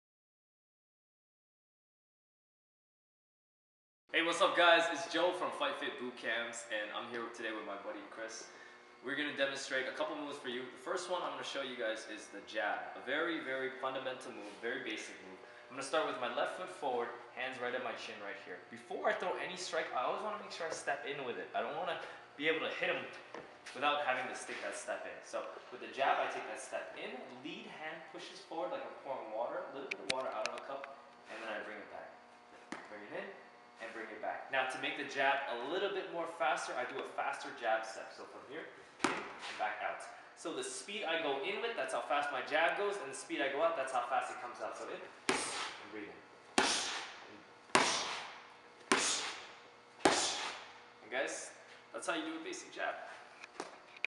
Kickboxing Basics How to Jab
shows you a basic jab.

Great beginner strike!

to start today